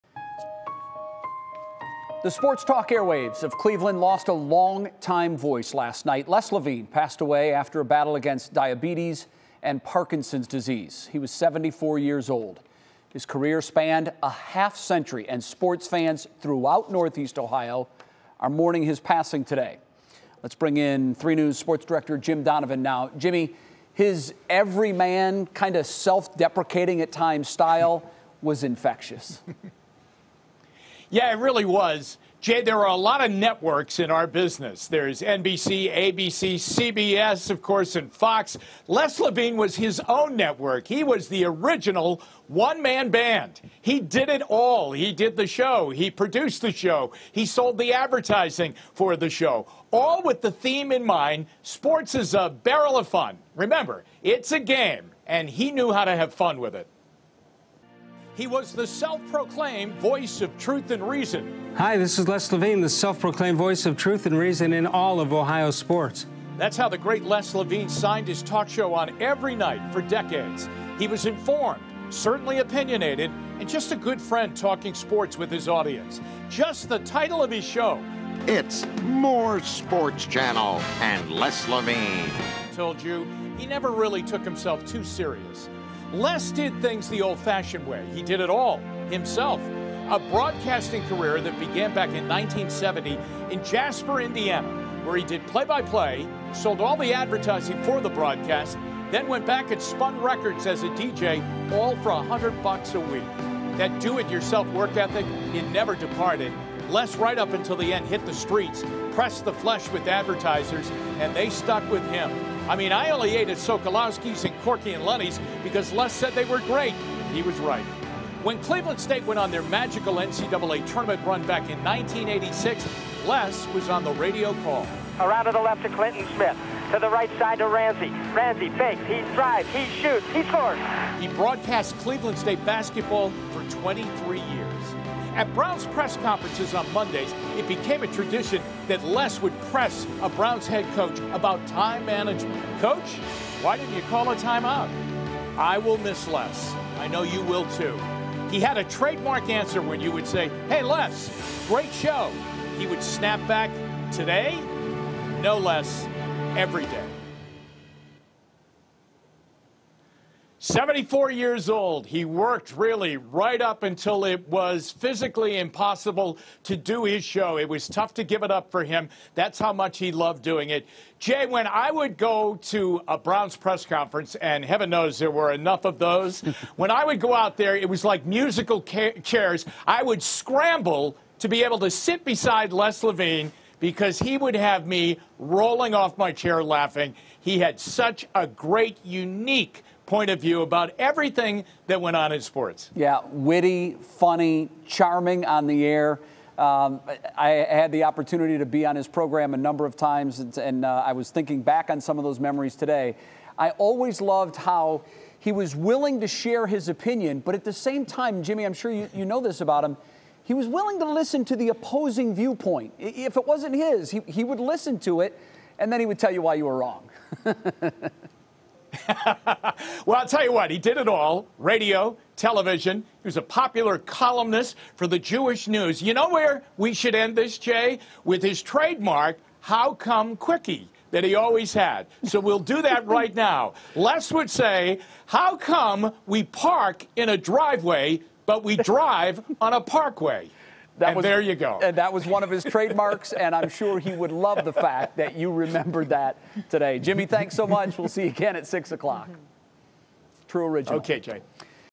Jim Donovan remembers the 'Voice of Truth and Reason' Les Levine
Levine had been battling Parkinson’s disease for several years, but continued to work until late December.  Jim Donovan has more on this Cleveland radio and TV icon.